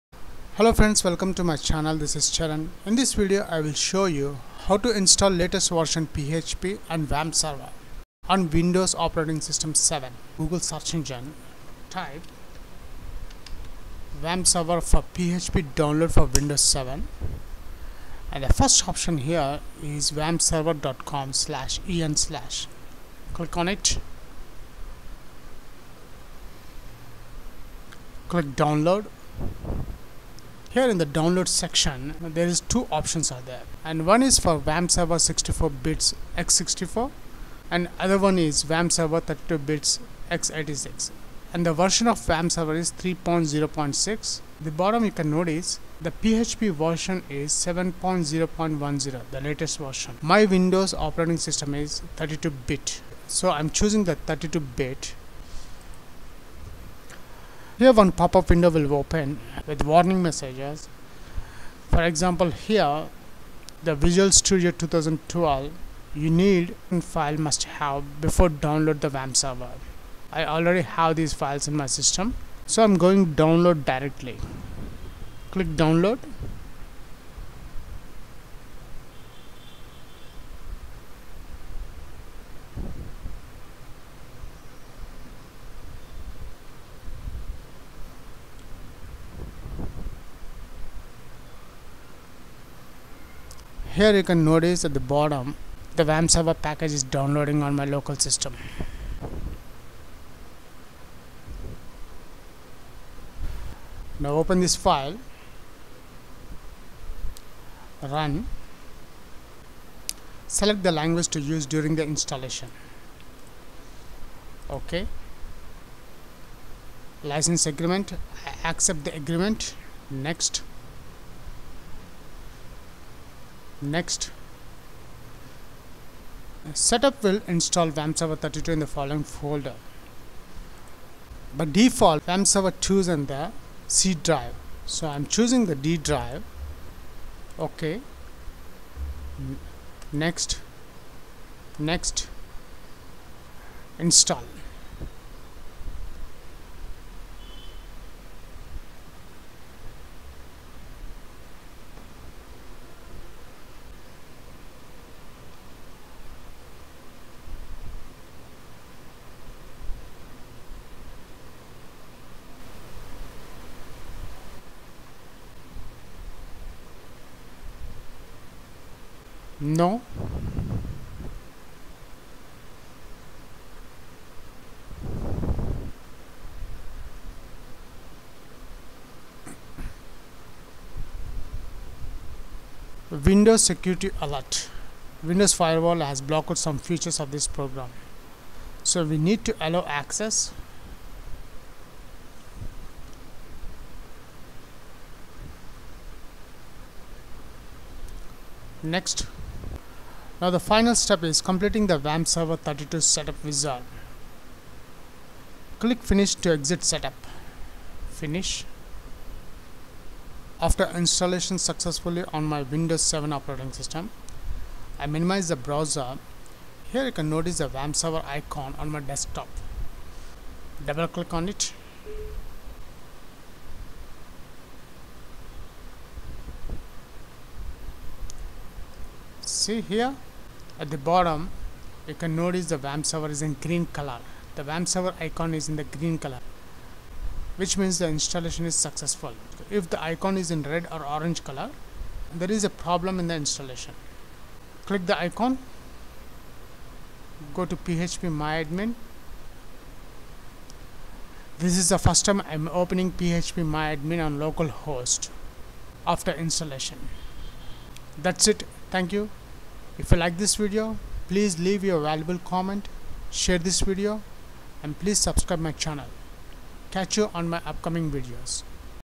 WAMPServer 3.0.6 (PHP 7.0.10) Install Safe On 32 Bits Windows 7
A tutorial to download safely Apache WAMPServer PHP latest version on windows 7